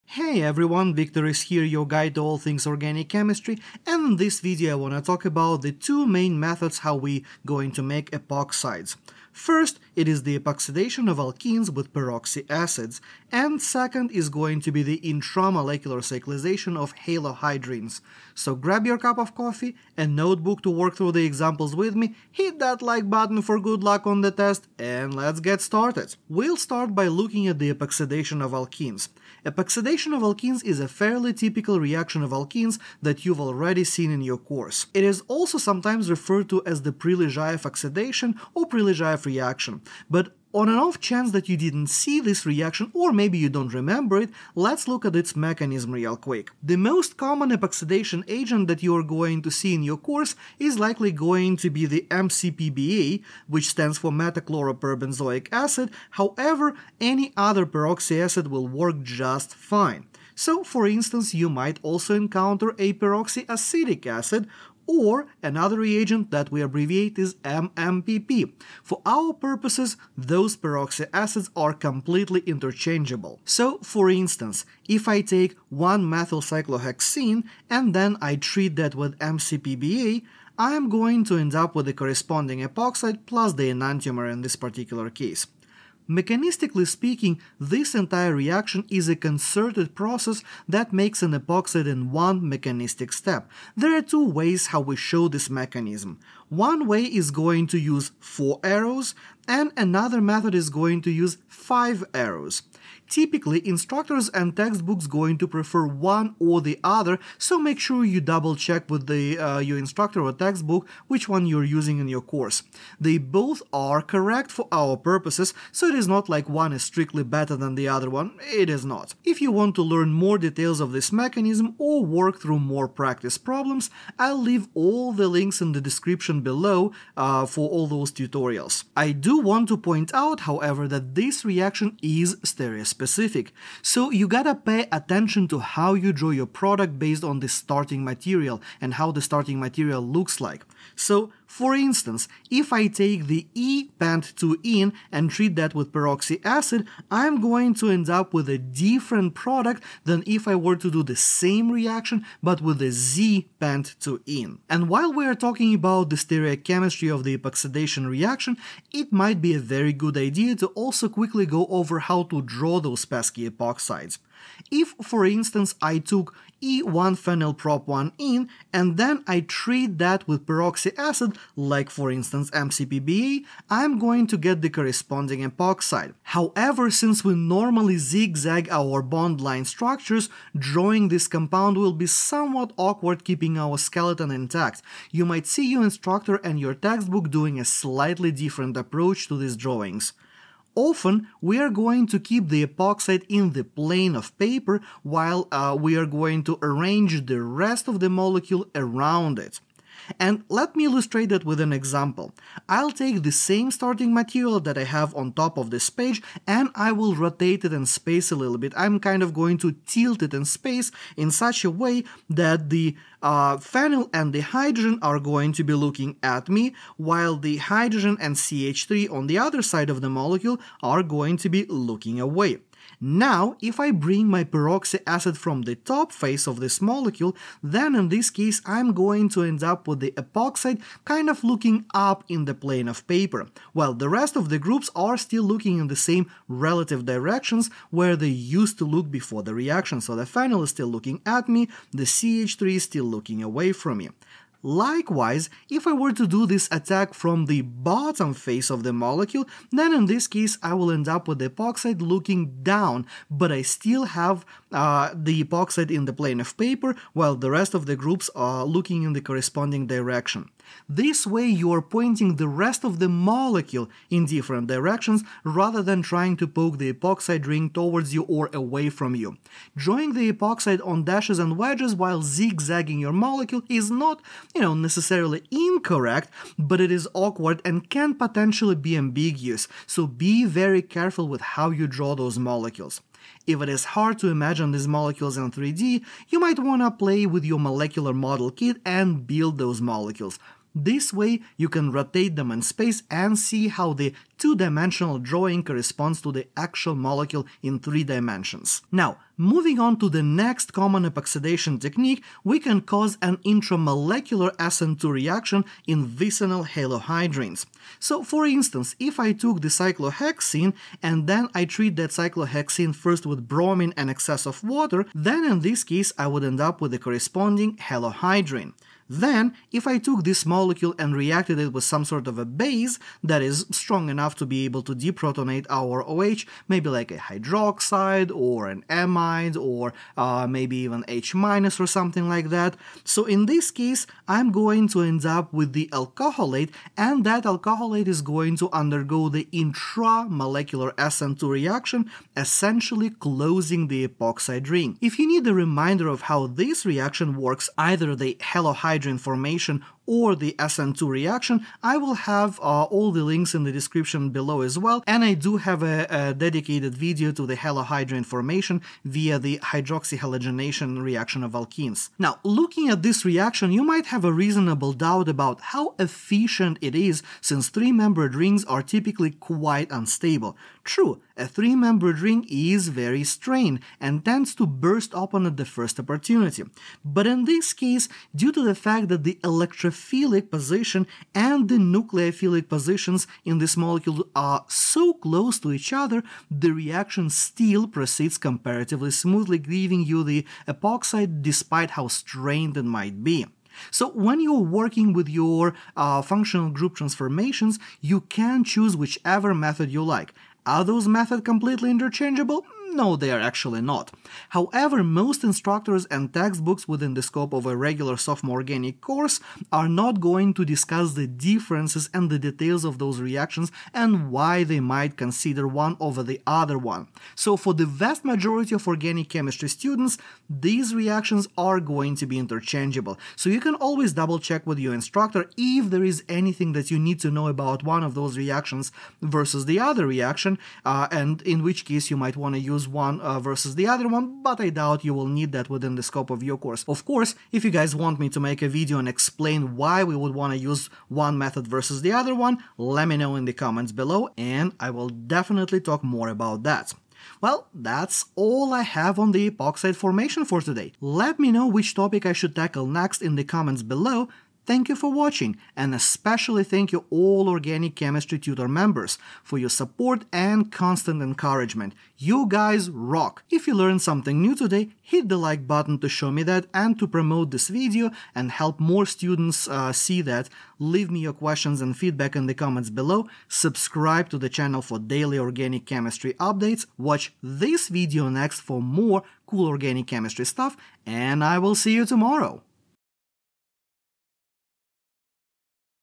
Synthesis of Epoxides via Epoxidation with mCPBA and Halohydrins
Synthesis of epoxides is a useful synthetic procedure that you might need to use in your course. In this tutorial I'll discuss the two main methods of the epoxide synthesis: epoxide formation via epoxidation of alkenes with mCPBA and other peroxy acids, and formation of epoxides via halohydrins.

00:00 Intro
00:26 Epoxidation of Alkenes
02:34 How to Draw Epoxides
04:58 Cyclization of Halohydrins